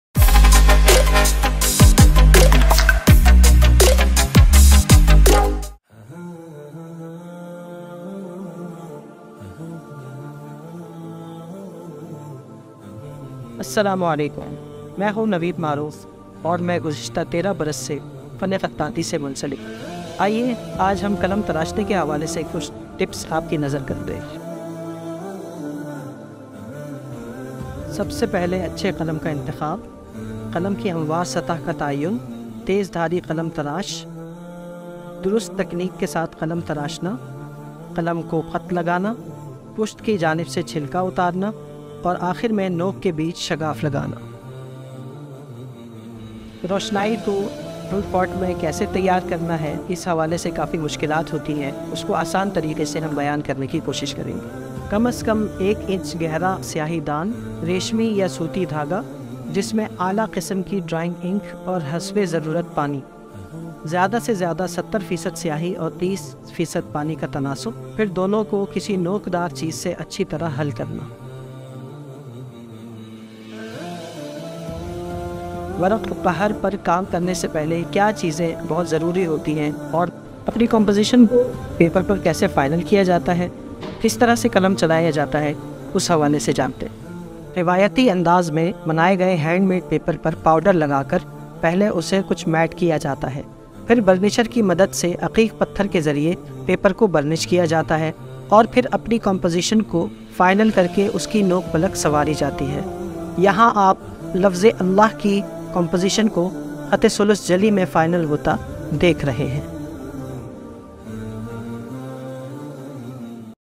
Basic Calligraphy Tips
How to make pen (qalm), prepare ink and paper